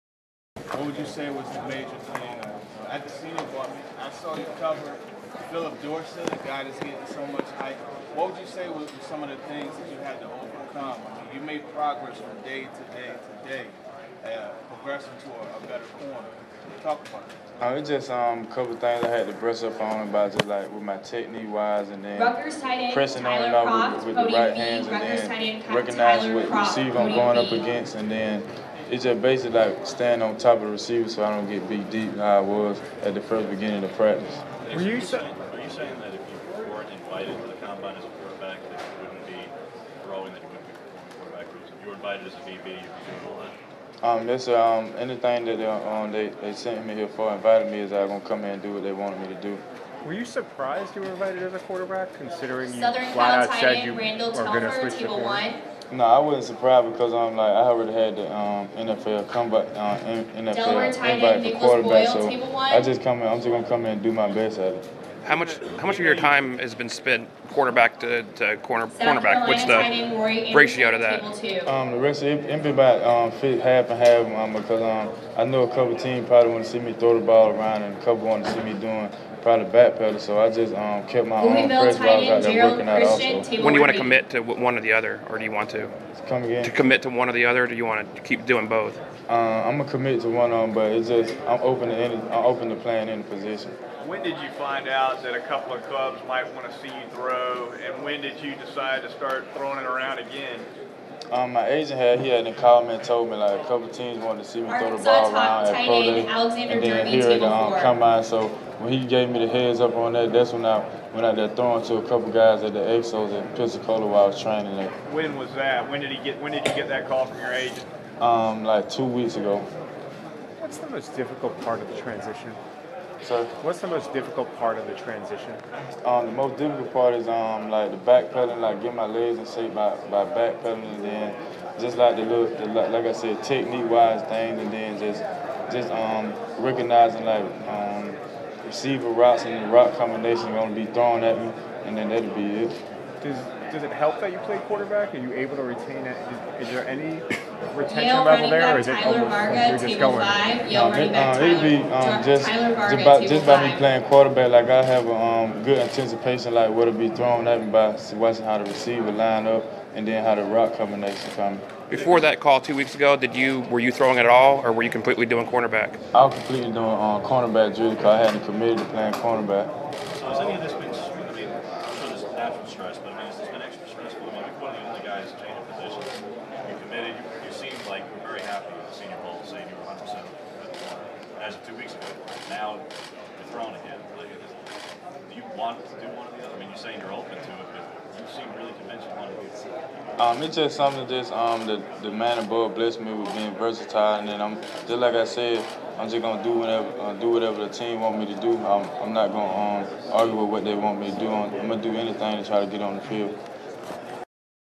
Nick Marshall on decision to play QB at 2015 NFL Combine
Nick Marshall discusses practicing both quarterback and cornerback before NFL Combine.